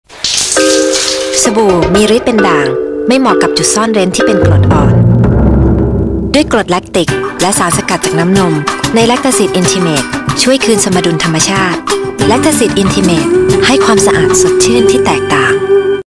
Lactacyd Intimate Cleansing "Soap" 15s - Thailand, 2002
โฆษณา แลคตาซิด อินทิเมท ทำความสะอาดจุดซ่อนเร้น ปีพ.ศ. 2545